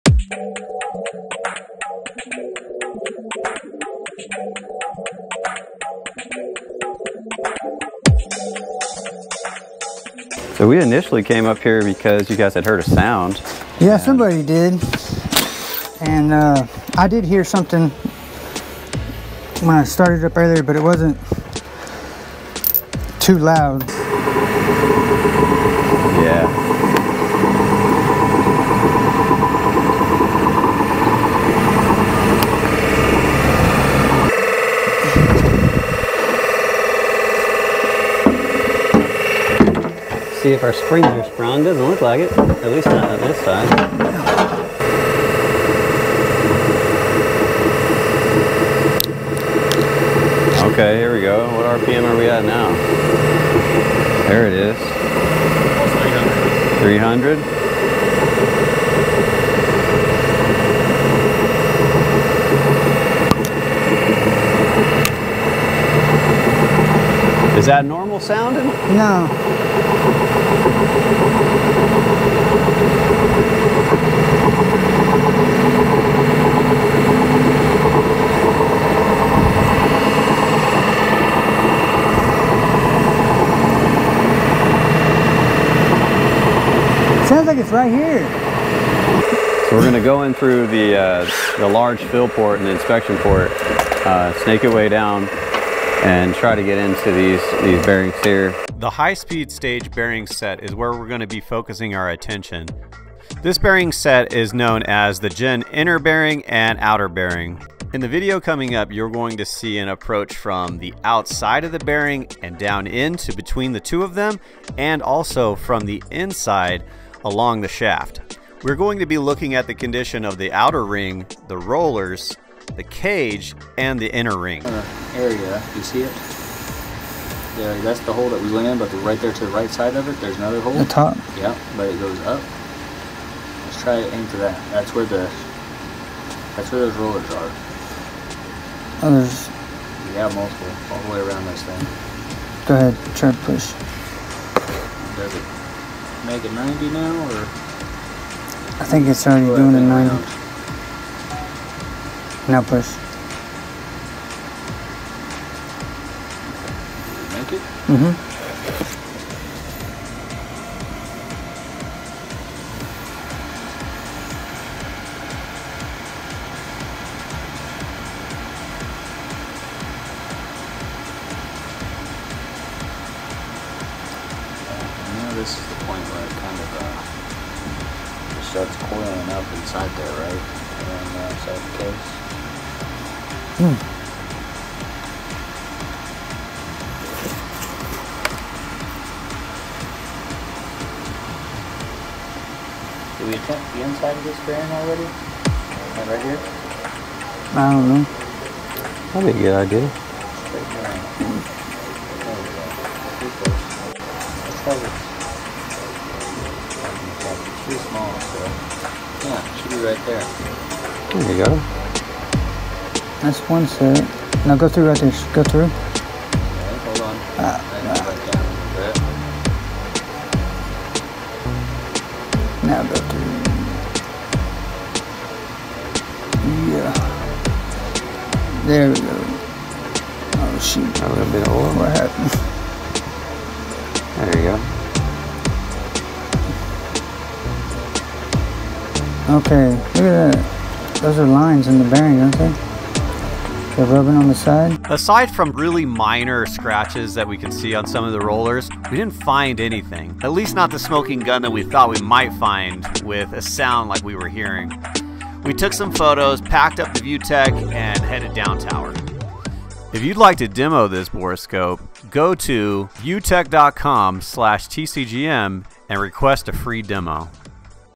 Wind Turbine Gearbox Inspection MM92 Borescope
MM92 wind turbine gearbox borescope inspection using the ViewTech VJ4.

If you would like to receive a free demo of the ViewTech Borescope here is the link.

Wind Turbine Technician, Wind Tech Day in the life, Wind Energy Career